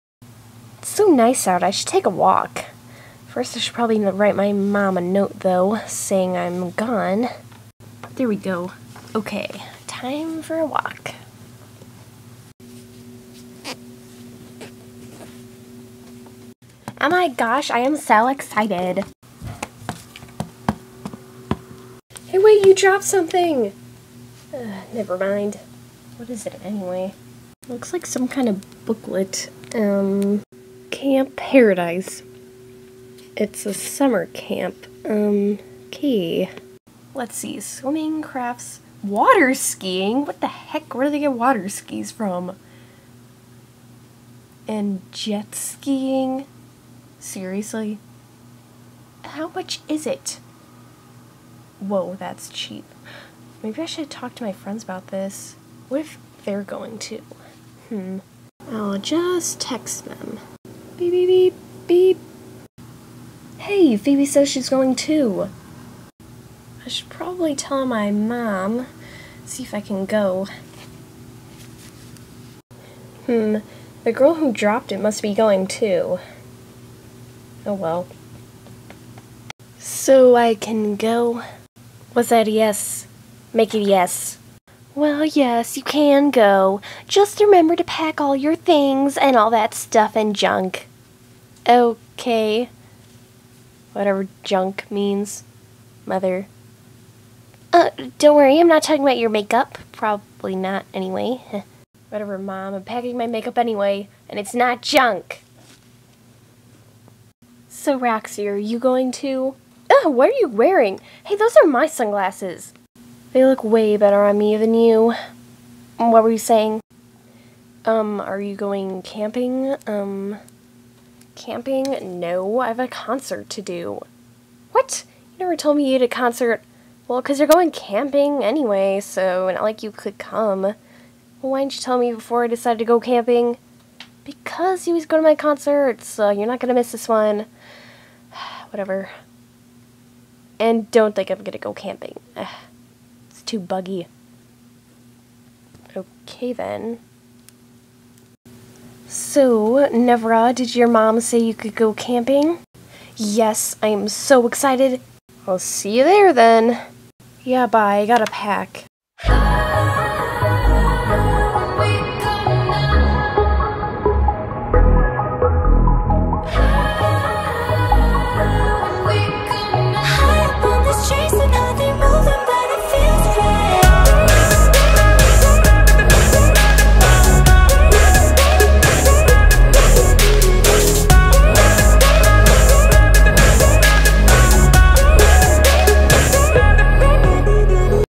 Bratz- Camp Paradise Ep 1
Nevra goes for a walk and finds a pamphlet for "Camp Paradise," Phoebe tells her sister she is going there too.